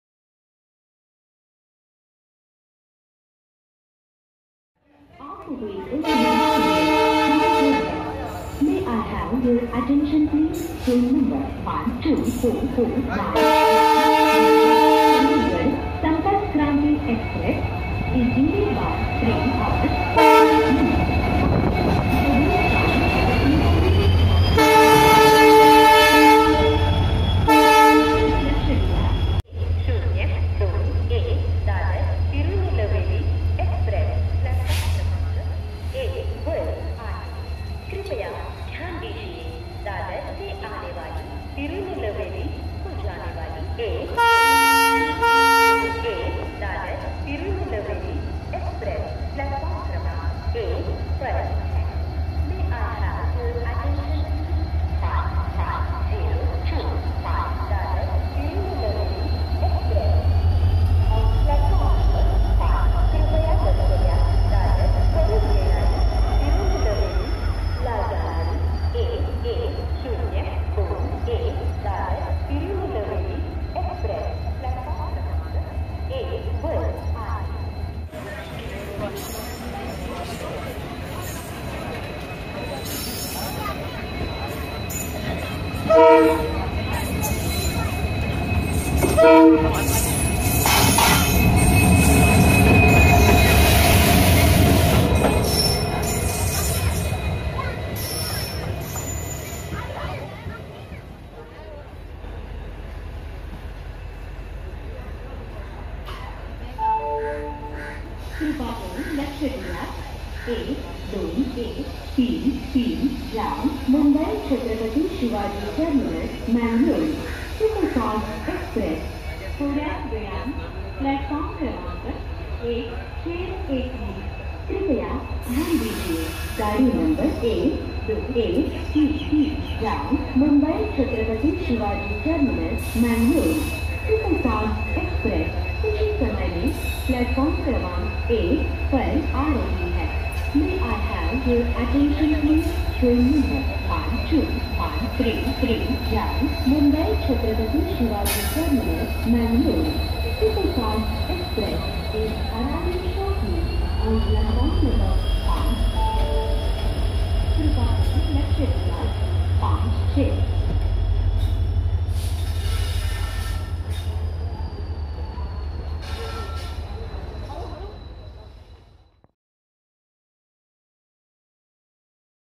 light locomotive : EMD of India Railway
This are the most powerful locomotives of India Railway.
1. Golden rock WDP-4B

2. Pune WDG4

3. KJM WDP-4

4. Gooty WDG4

The Rail Zone By Saiesh »KONKAN RAIL ZONE